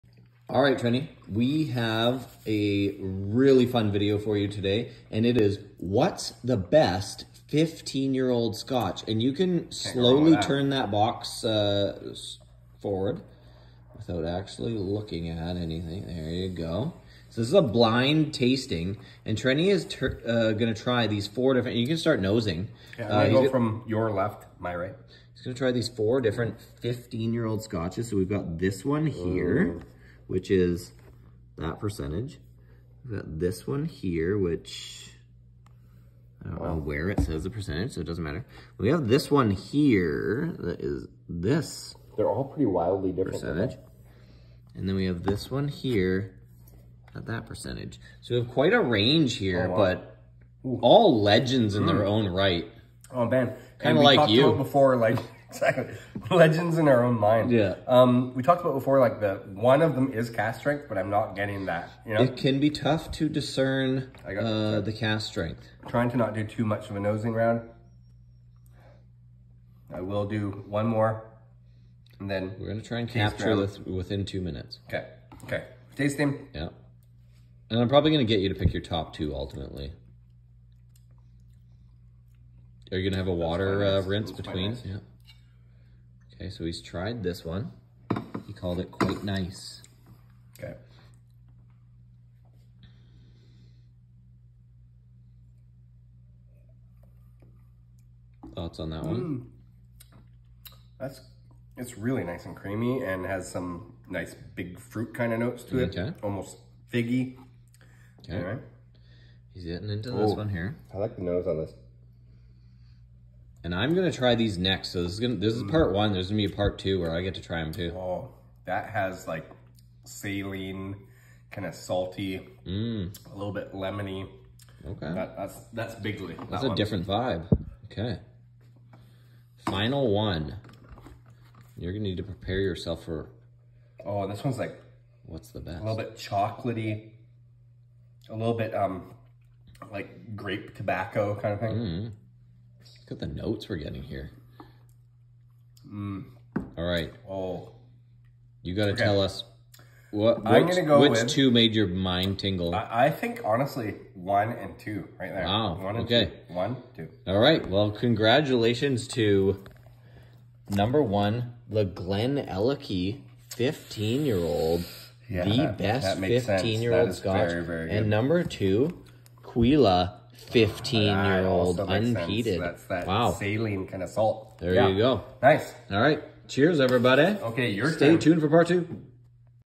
WHAT’S THE BEST 15 YEAR OLD SCOTCH? - BLIND TASTING P.1
BLIND TASTING REVIEW - WHAT’S THE BEST 15 YEAR OLD SCOTCH?   Part1 featuring Glendronach 15 Glenfiddich 15 Caol Ila 15 and Glenallachie 15